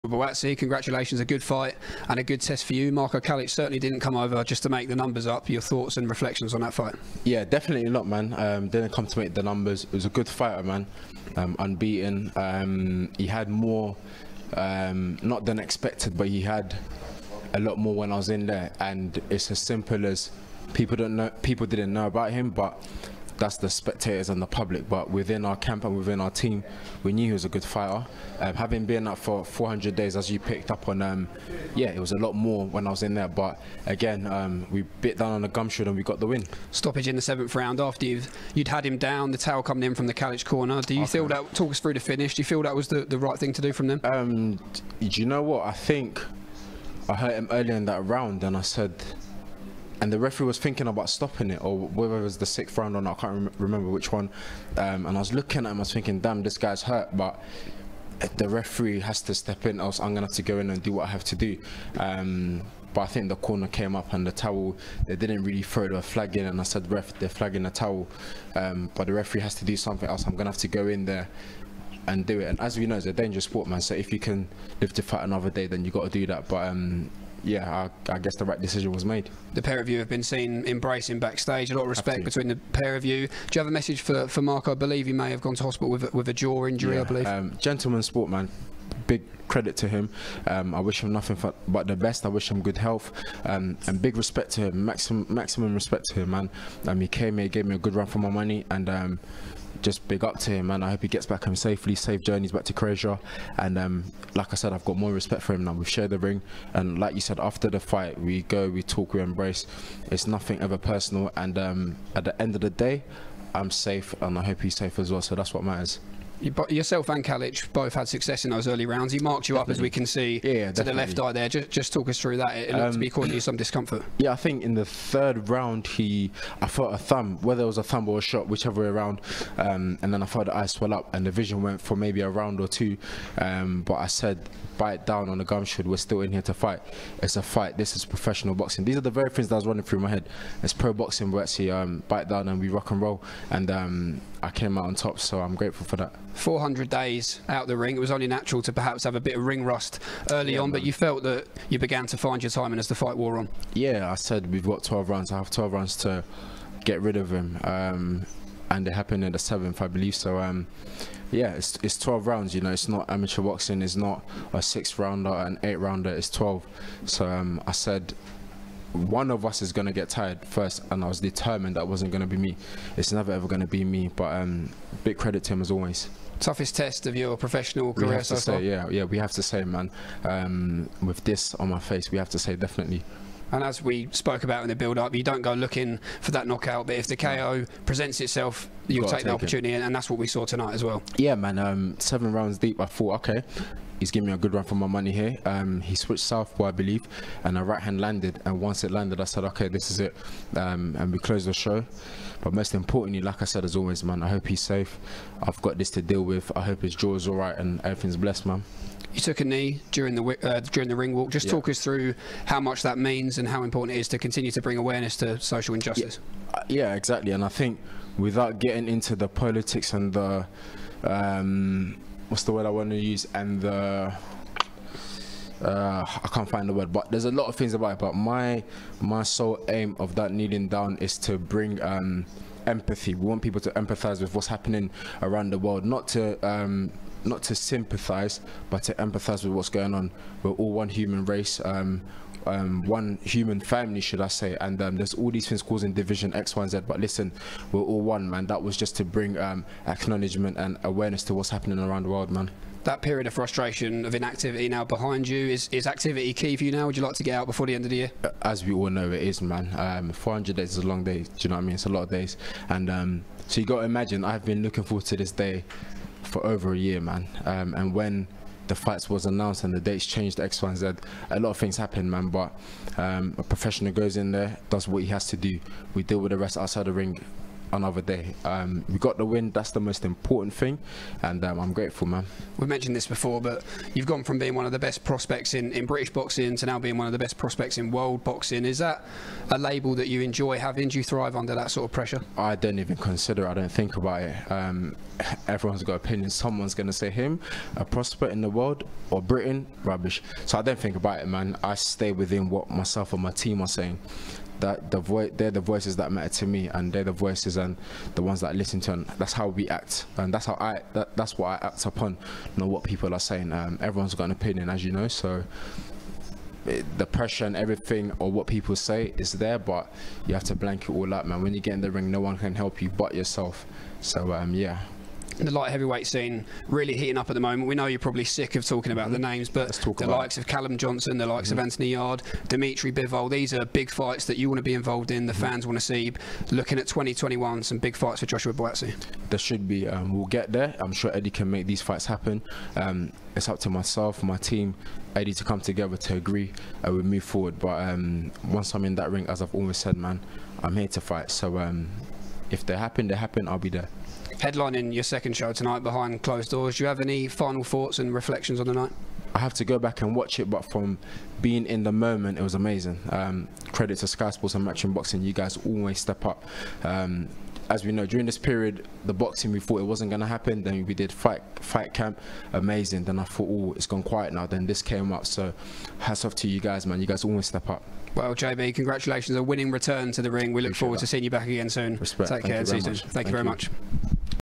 “Calic gave me a run for my money!” - Joshua Buatsi moves 13-0 after 400 days out of ring
Joshua Buatsi came through his toughest test to date with a seventh round stoppage over Marco Calic in Milton Keynes. It's a big 2021 coming up for JB